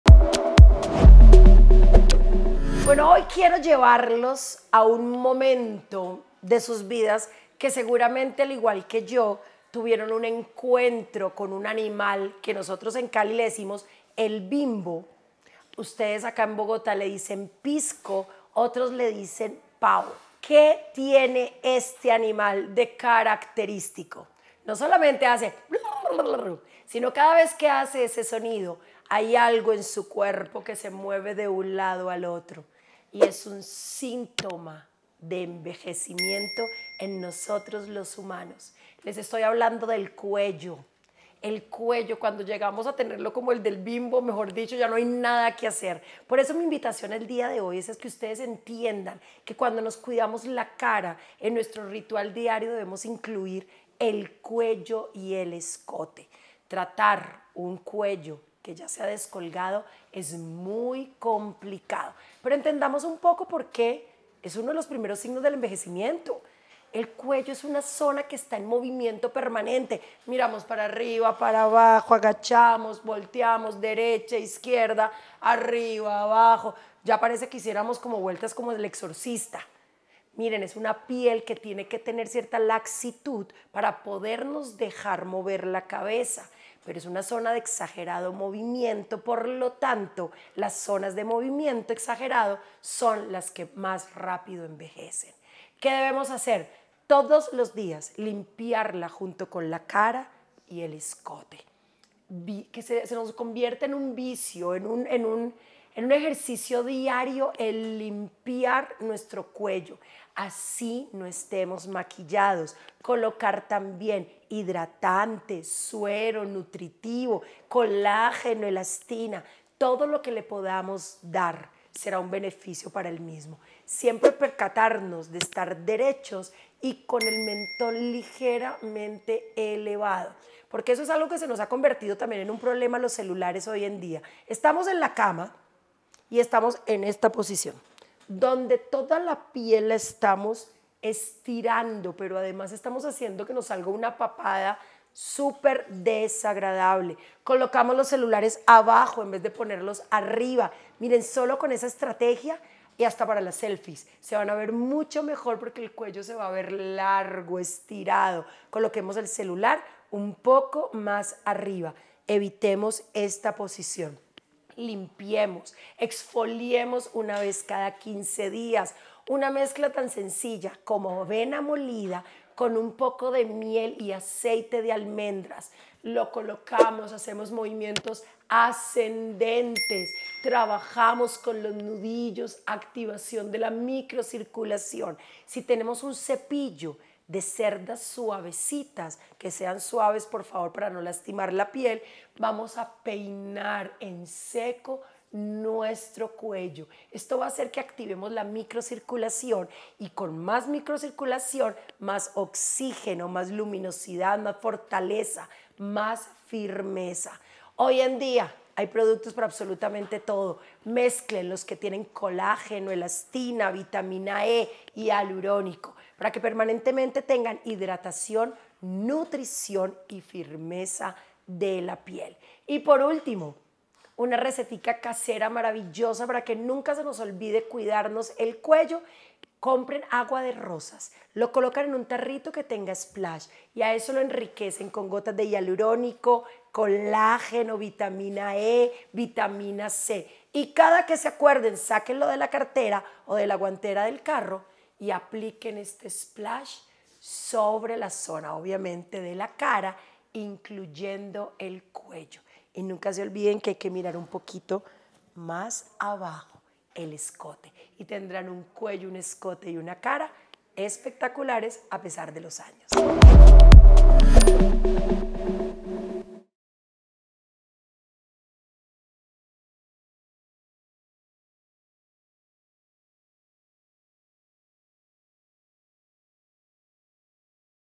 ¿Por qué cuidar de la piel del cuello?
KienyKe.com consultó a la Dra. Alexandra Rada sobre la importancia de cuidar la piel del cuello.